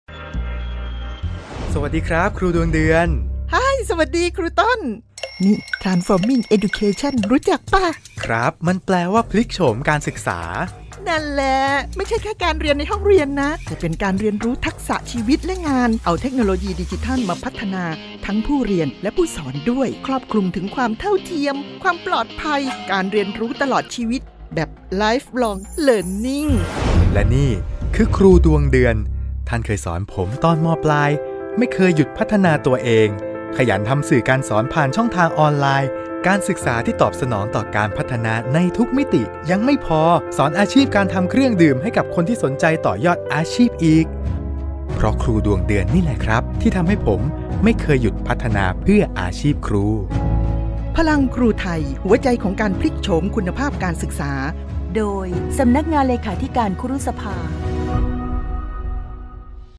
ทรานฟอร์มทีชเชอร์ : รางวัลรองชนะเลิศอันดับ 2 สปอตวิทยุวันครู 2566
:: รางวัลรองชนะเลิศอันดับ 2 สปอตวิทยุวันครู ::
การประกวดสปอตวิทยุ และสปอตโทรทัศน์ "วันครู" ประจำปี 2566
ในหัวข้อ “พลังครูไทย: หัวใจของการพลิกโฉมคุณภาพการศึกษา”

ผลงานโดย :  24 Working

เพื่อสร้างกำลังใจให้ครูไทยและปลุกวิญญาณความเป็นครู ผ่านแบบอย่างครูที่ดีผู้เป็นครูต้นแบบ ดังคำที่ว่า (ตัวอย่างที่ดีมีค่ามากกว่าคำสอน) เสมือนการส่งไม้ต่อจากครูรุ่นเก๋าสู่ครูรุ่นใหม่ ให้พัฒนาตนเองในสายวิชาชีพเพื่อผู้เรียนอย่างแท้จริง โดยใช้สื่อเทคโนโลยีคู่ขนานไปพร้อมๆกับนโยบายการพัฒนาการศึกษาตามกระทรวงศึกษาเป็นผู้ให้แนวทาง

สำนักงานเลขาธิการคุรุสภาได้จัดการประกวดสปอตวิทยุ และสปอตโทรทัศน์ "วันครู" ประจำปี 2566
เพื่อรณรงค์ ส่งเสริม สนับสนุนให้เห็นคุณค่าของครู รวมทั้ง สร้างสำนึกในพระคุณของครู ด้วยการผลิตสื่อในแนวทางสร้างสรรค์ในรูปแบบของสปอตวิทยุ และสปอตโทรทัศน์